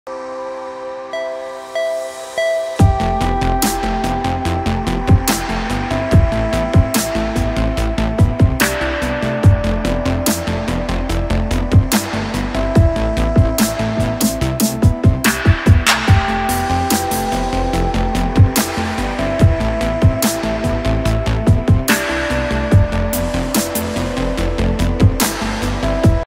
Utah Mountains in snow
Jetskiing on Utah Lake on December 26th, 2019.